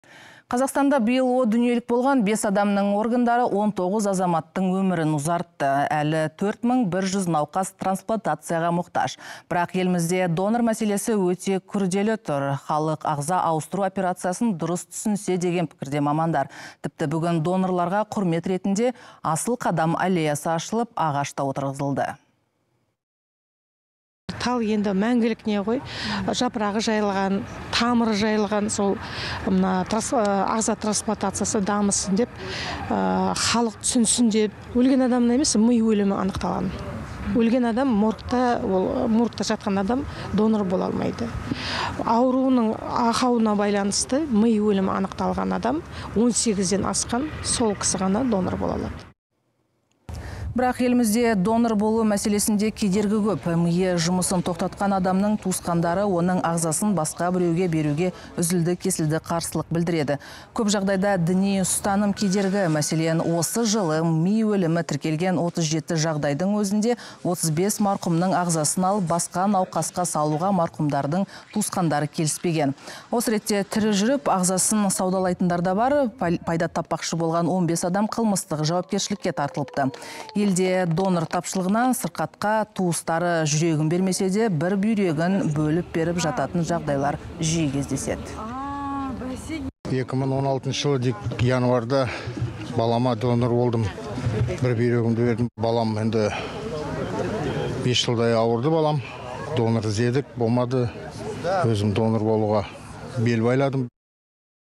Биыл елімізде қайтыс болған бес адамның ағзасы 19 адамның өмірін ұзартты
Қазақстанда биыл қайтыс болған бес адамның органдары 19 адамның өмірін ұзартты. Әлі 4100 науқас трансплантацияға мұқтаж. Дгенмен елімізде донор мәселесі өте күрделі. Мамандар ағза ауыстыру операциясын дұрыс түсінсе деген пікірде. Тіпті бүгін донорларға құрмет ретінде «Асыл қадам» аллеясын ашып, ағаш та отырғызды.